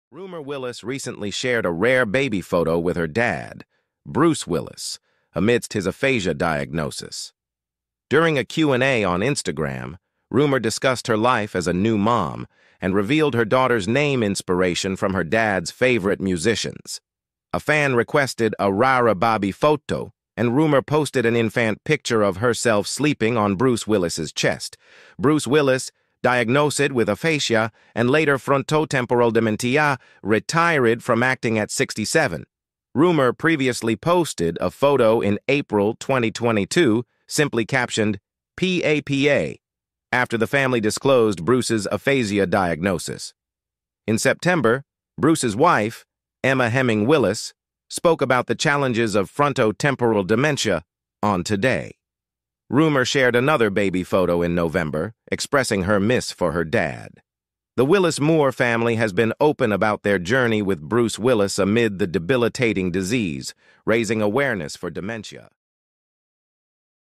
Rumer Willis Recently Shared a Rare Baby Photo With Her Dad
Rumer Willis recently shared a rare baby photo with her dad, Bruce Willis, amidst his aphasia diagnosis.
During a Q&A on Instagram, Rumer discussed her life as a new mom and revealed her daughter's name inspiration from her dad's favorite musicians.
A fan requested a rare baby photo, and Rumer posted an infant picture of herself sleeping on Bruce Willis's chest.
Bruce Willis, diagnosed with aphasia and later frontotemporal dementia, retired from acting at 67.
Rumer previously posted a photo in April 2022 simply captioned "P A P A" after the family disclosed Bruce's aphasia diagnosis.
In September, Bruce's wife, Emma Heming Willis, spoke about the challenges of frontotemporal dementia on TODAY.
Rumer shared another baby photo in November, expressing her miss for her dad.
The Willis/Moore family has been open about their journey with Bruce Willis amid the debilitating disease, raising awareness for dementia.

Video Voice                     -  Voice From Elevenlabs.io website
Video Information          - From Google
Video/Image Present    - Googles & Instagram